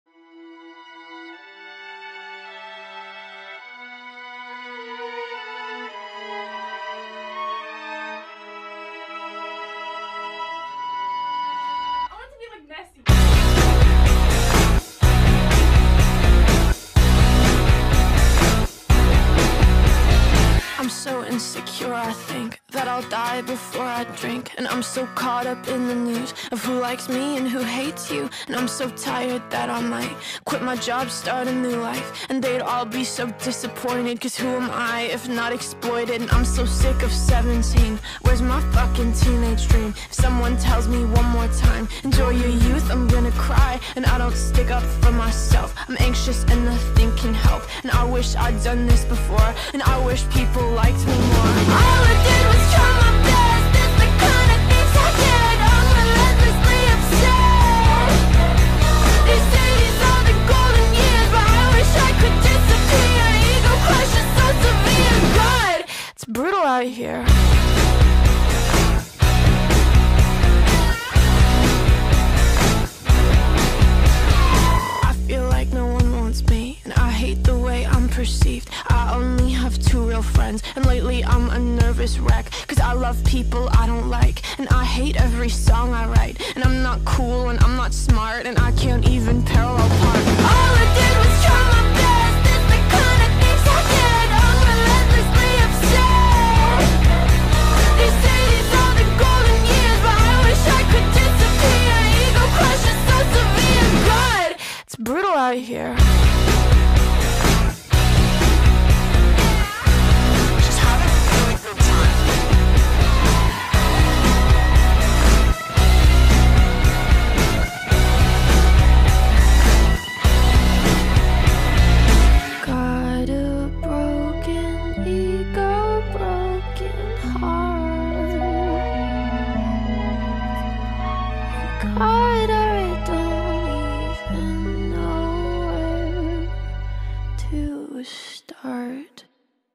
Brutal Mep | Gacha Audition Mep | CLOSED 15/15 | Read desc
Fourth mep I’ve hosted, please don’t fail!!

(Unlike previously on my Meps, I made sure each part Is equally spaced)

[this is an audition mep, meaning you don’t have to ask for a part. Go ahead and make one! Keep in mind you may not be chosen]

Theme: teenage stress

Rules:
1. No NSFW (18+)
2. Gore is allowed, but not extreme.
3. Please use cut in and cut out
4. No slideshows
5. Don’t use your watermark, I’ll put one in for you.
6. Due date is when all parts are filled
7. Only one part per person
8: Use the tag BrutalFish on your part.
9: You can Audition for multiple parts, but you will only be given one
10. Fandom Is allowed but OCs are preferred!

More rules may be added

Parts
☀️ : taken

Intro: me ☀️

Other info